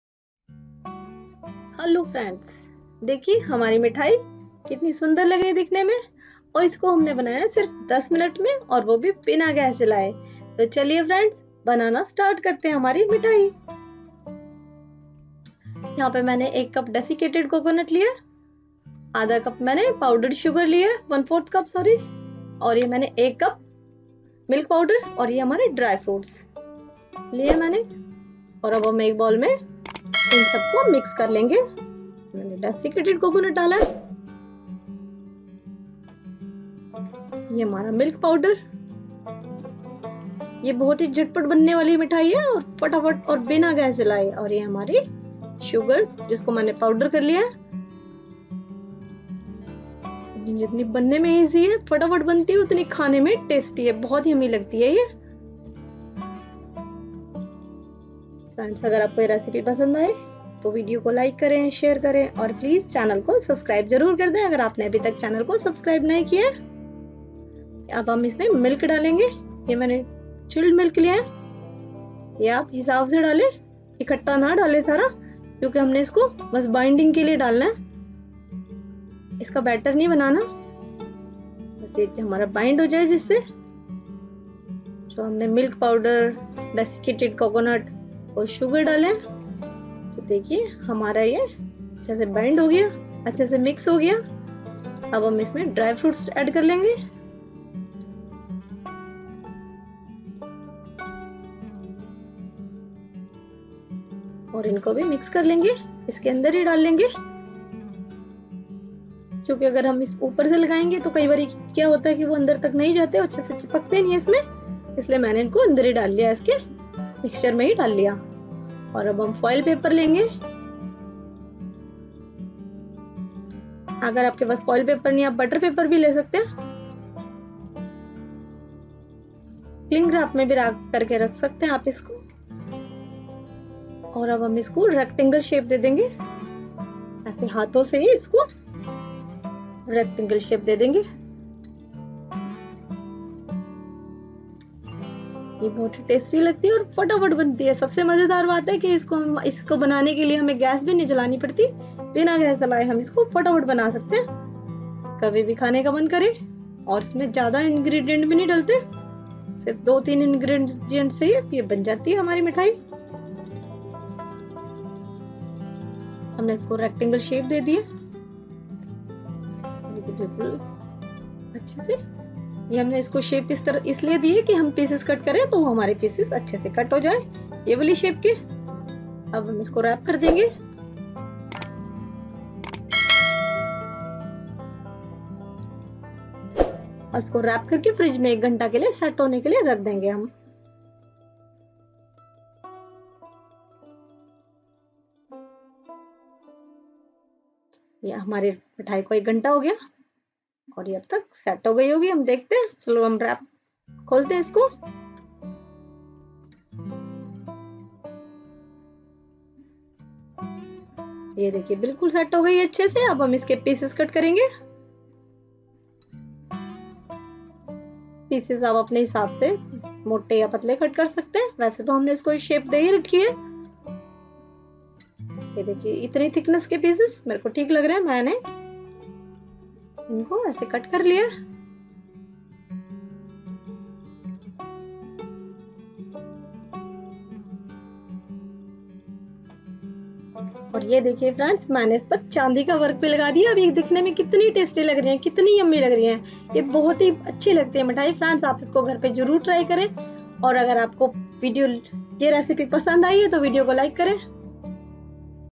बिना गैस जलाए 5 मिनट में स्वादिष्ट मिठाई | ना चाशनी | vrat ki recipe in hindi
With Regards,
Masala Kitchen Recipes
Like_ Share_Subscribe